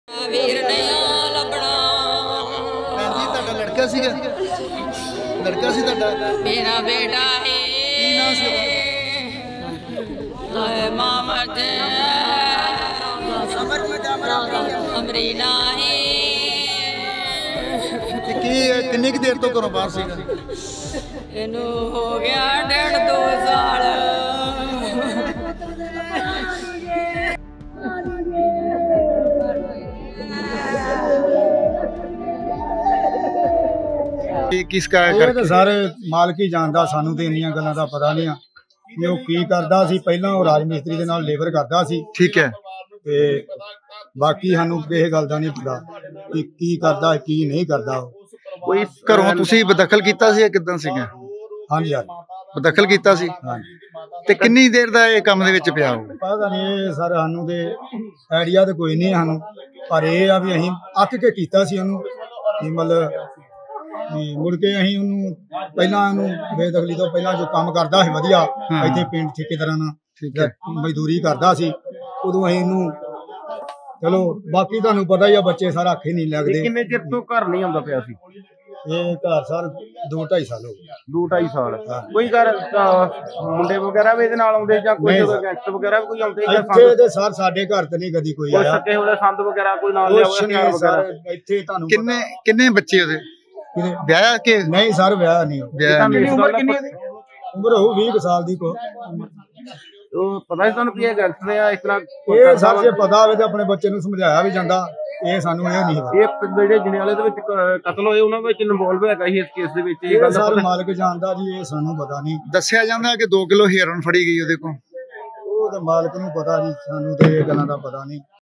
ਐਨਕਾਊਂਟਰ 'ਚ ਮਾਰੇ ਗੈਂਗਸਟਰ ਦੇ ਦੇਖੋ ਘਰ ਦੇ ਹਾਲਾਤ, ਚਾਰੇ ਪਾਸੇ ਪੈ ਰਿਹਾ ਚੀਕ-ਚਿਹਾੜਾ
Punjabi Bulletin